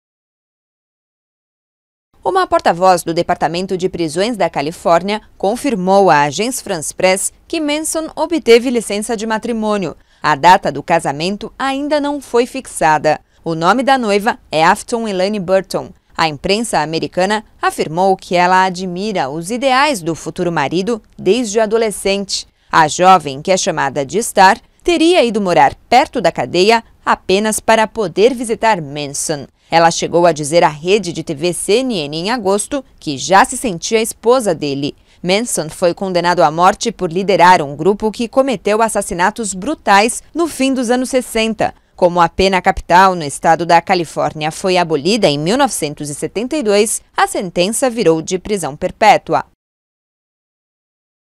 Charles Manson vai casar com mulher 54 anos mais nova
Conhecido como guru-assassino, Charles Manson, de 80 anos, vai casar em breve com uma moça de 26 anos. A cerimônia será realizada na prisão. Manson cumpre prisão perpétua por sete homicídios, entre eles o da atriz Sharon Tate. A mulher do diretor Roman Polanski estava grávida de oito meses quando foi assassinada.